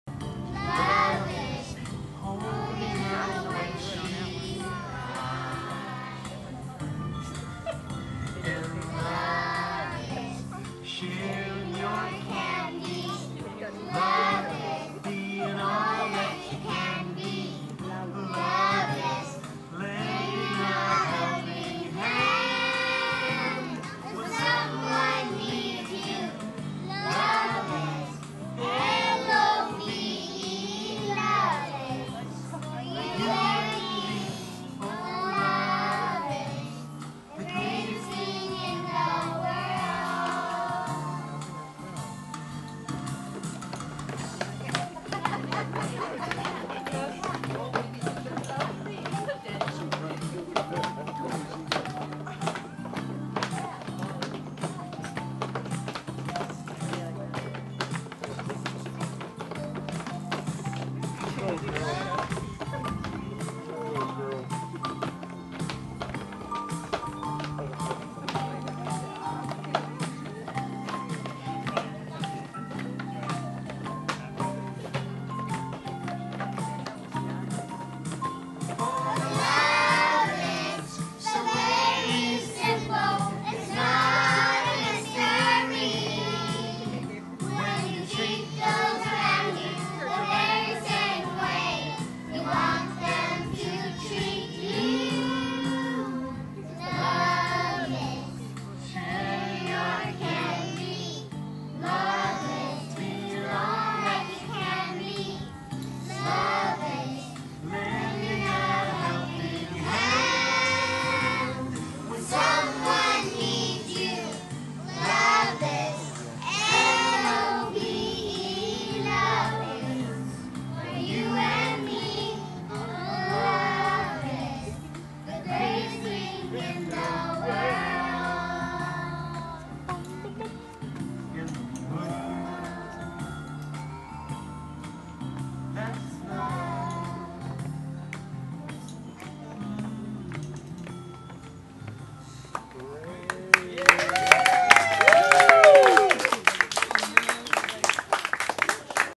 LB Preschool Presentation Part 2
Part 2: LB singing at Escalante Preschool Graduation for the soon the be Kindergarteners.  He did pretty good considering the other kids had been singing and practicing this for a couple of months and he only had a few days under his belt.  Gotta love the required attire of a jean jacket, jeans, and a white shirt.  Don't know who combed his hair either...but he's looking sharp.